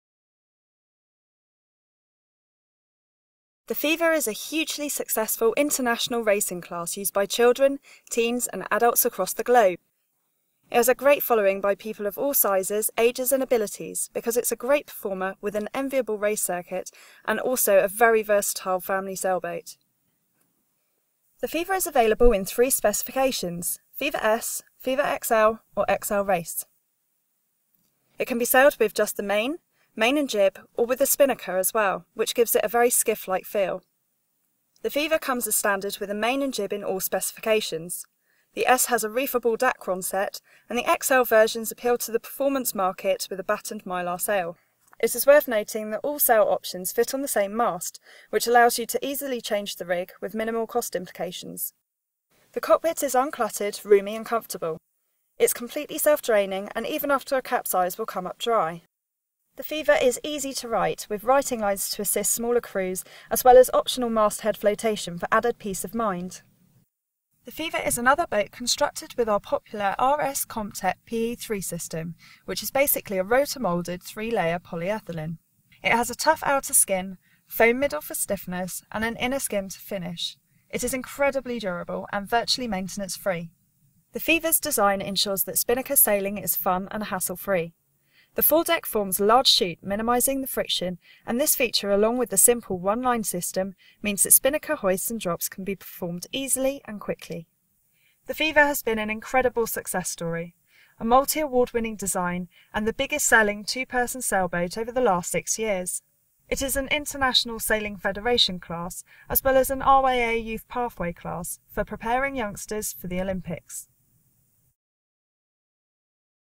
RS Feva Overview - RS Sailing's family sailing dinghy & youth racing boat
An introduction to the RS Feva - our international best selling 2 person sailboat!

The Feva is a hugely successful international racing class and ISAF class used by children, teens and adults across the globe.  It has a great following by people of all sizes because it's a great performer with an enviable race circuit but also is a very versatile family sailboat.